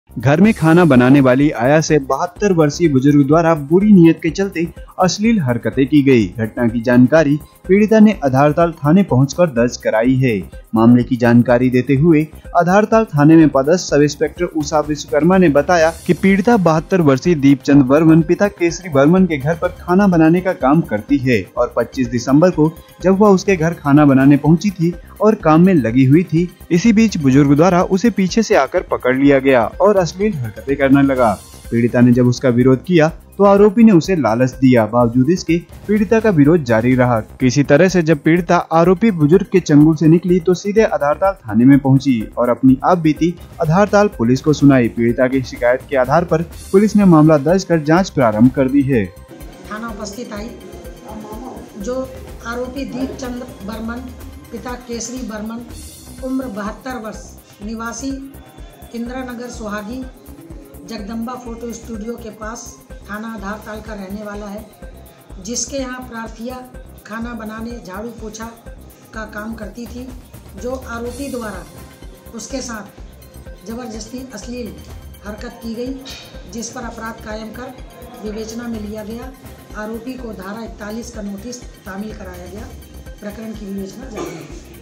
जबलपुर : घर में खाना बनाने वाली आया से 72 वर्षीय बुजुर्ग द्वारा बुरी नियत के चलते अश्लील हरकत की गई
घर में खाना बनाने वाली आया से 72 वर्षीय बुजुर्ग द्वारा बुरी नियत के चलते अश्लील हरकतें की गई। घटना की जानकारी पीड़िता ने अधारताल थाने पहुंचकर दर्ज कराई है। मामले की जानकारी देते हुए अधारताल थाने में पदस्थ सब इंस्पेक्टर उषा विश्वकर्मा ने बताया कि वह 72 वर्षीय दीपचंद बर्मन पिता केसरी बर्मन के घर पर खाना बनाने का काम करती है। और 25 दिसंबर को वह उसके घर खाना बनाने पहुंची थी और वह काम में लगी हुई थी। इसी बीच बुजुर्ग द्वारा उसे पीछे से आकर पकड़ लिया गया और अश्लील हरकतें करने लगा। पीड़िता ने जब इसका विरोध किया तो आरोपी उसे लालच देने लगा। बावजूद इसके पीड़िता का विरोध जारी रहा। किसी तरह बचकर वह आरोपी बुजुर्ग के चंगुल से निकली तो सीधे अधारताल थाने पहुंची और अपनी आप बीती अधारताल पुलिस को सुनाई पीड़िता की शिकायत के आधार पर पुलिस ने मामला दर्ज कर जांच प्रारंभ कर दी है।